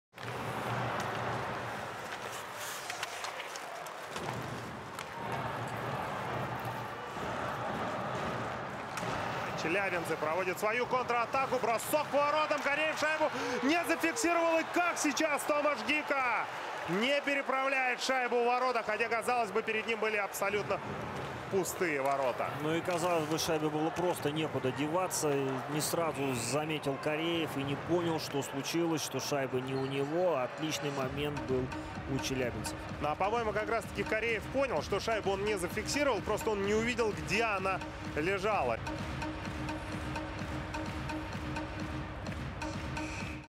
Tomas Hyka misses the empty net sensationally
Traktor forward Tomas Hyka misses the empty net